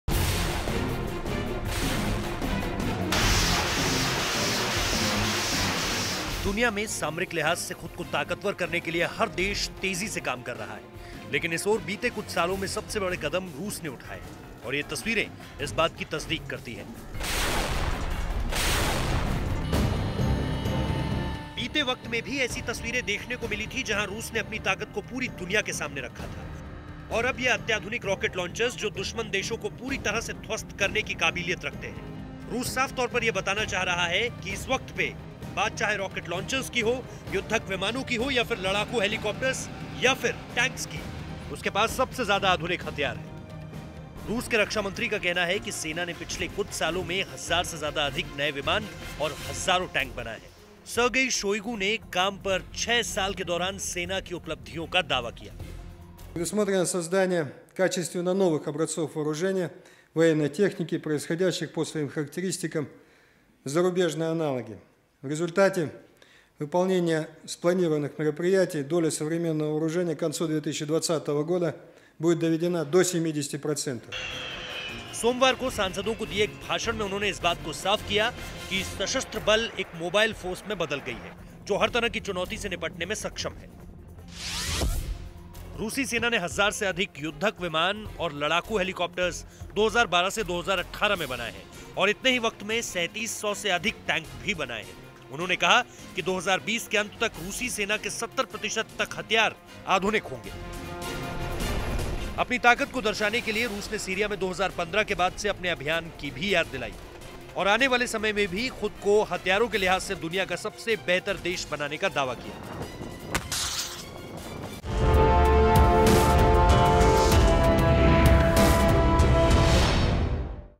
पूरी दुनिया को रूस ने दिखाई अपनी ताकत | Duniya Tak
दुनिया में समारिक लिहाज से खुद को ताकतवर करने के लिए हर देश तेजी से काम कर रहा है, लेकिन इस ओर बीते कुछ सालों में सबसे बड़े कदम रूस ने उठाए हैं और य़े तस्वीरें इस बात की तस्दीक करती हैं